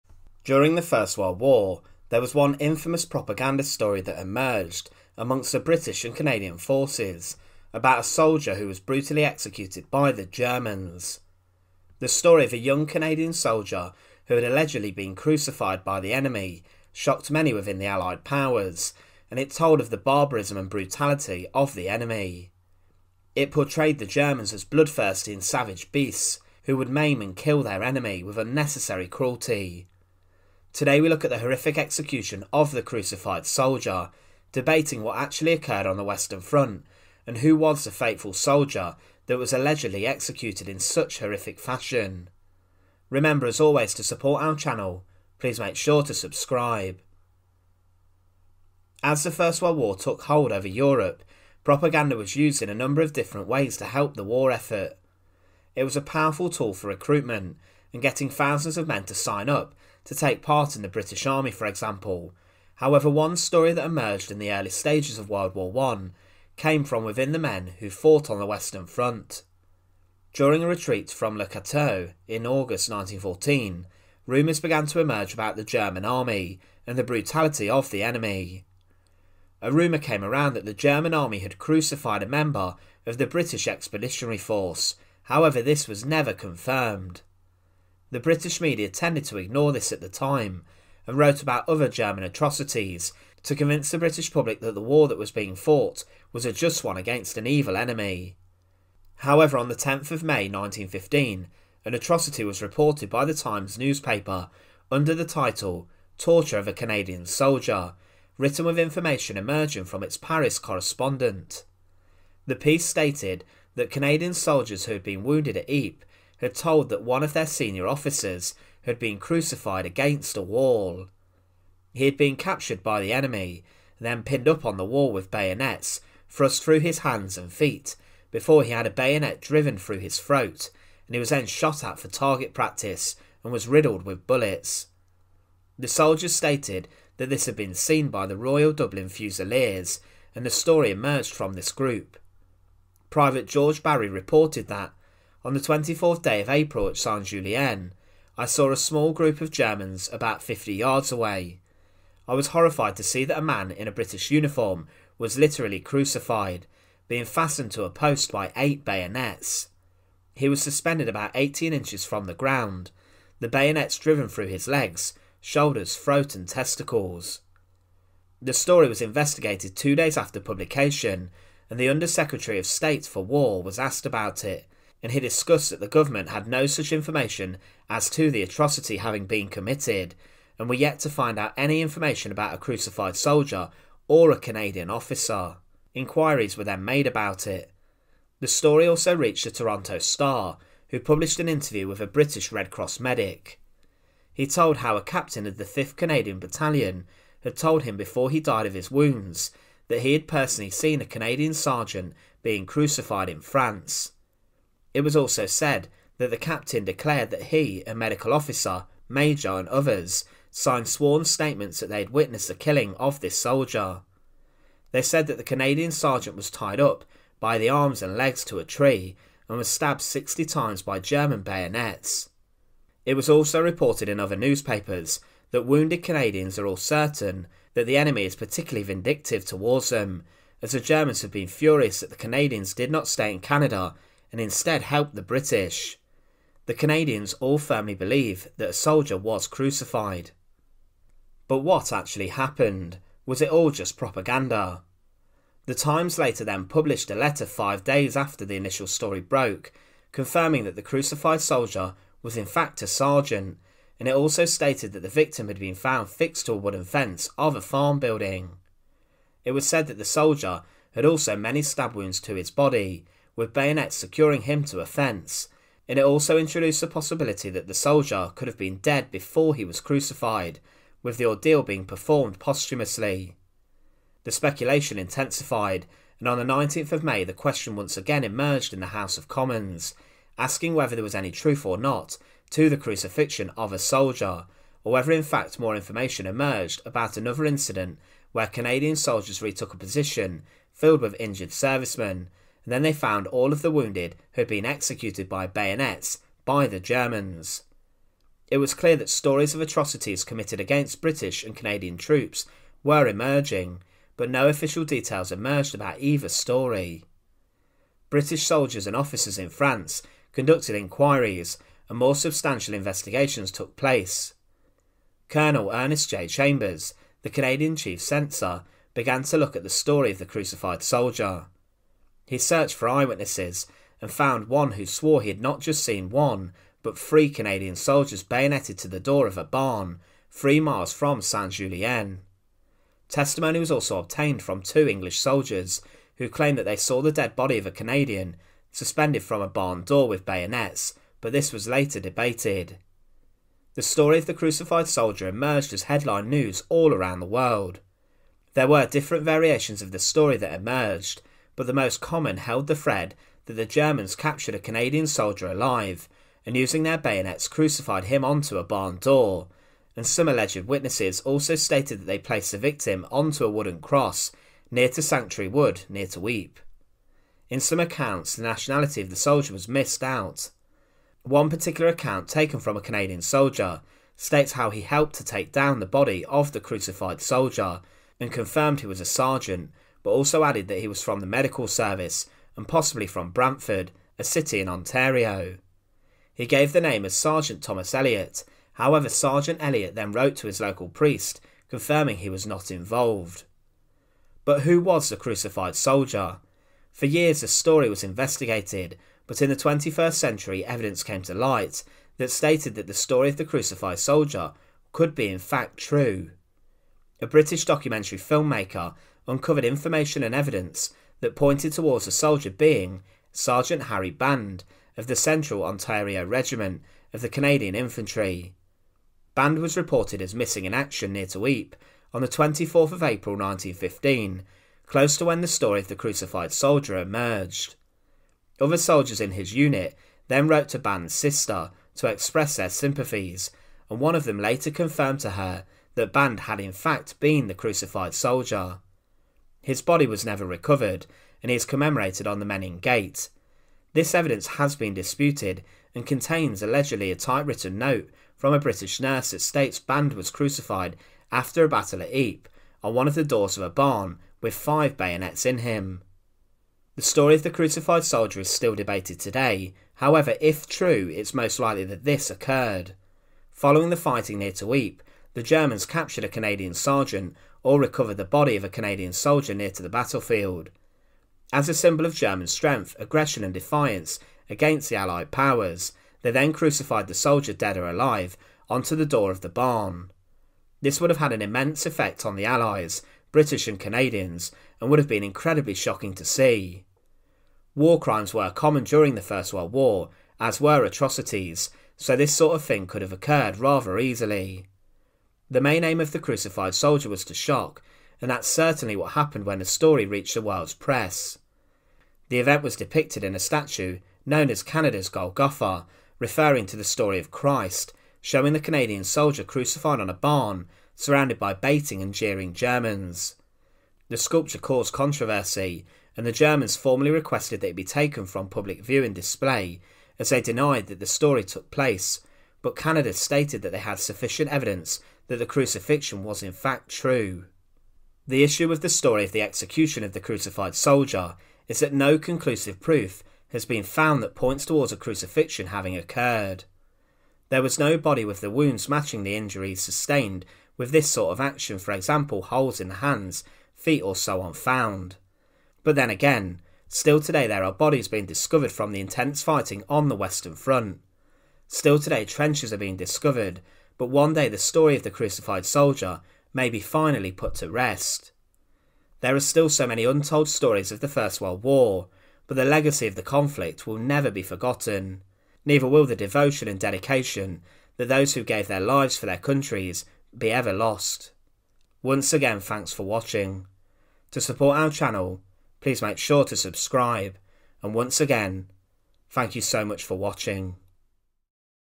The HORRIFIC Execution Of The CRUCIFIED Soldier Of World War 1
During the First World War, there was a story that travelled around the Western Front and the Trenches that struck fear into the hearts of the allied nations. Rumours emerged that the German Army near to Ypres had killed a Canadian soldier in barbaric fashion. It was alleged that this Canadian sergeant had been captured following the intense fighting, and then was on the door of a barn crucified and nailed to the wooden structure using bayonets. This story terrified many, and made people think that the German enemy were incredibly cruel and un-necessarily savage. The media used this story as propaganda and this helped people to think that the war was a just one, a crusade against a savage enemy. But was it true?

The story of the crucified soldier emerged in 1915, after the intense fighting at Ypres. It was investigated a number of times, and was even subject to debate inside of Parliament. Some incredibly interesting evidence emerged and many soldiers from different regiments claimed that they even saw the body of a crucified soldier pinned to a wooden structure, and some even said he was found on a wooden cross. During investigations in the 21st century, a journalist turned up evidence that allegedly even named the crucified soldier, however a body has still yet to be found.

Many stories of World War 1 still remain untold, and still today many bodies are being uncovered from the battlefields and interred with the dignity they deserve. But the story of the crucified soldier is one of those stories, and who is to say that as to years go on, the story of the crucified soldier could be told for the world.

So join us today as we look at, 'The HORRIFIC Execution Of The Crucified Soldier.'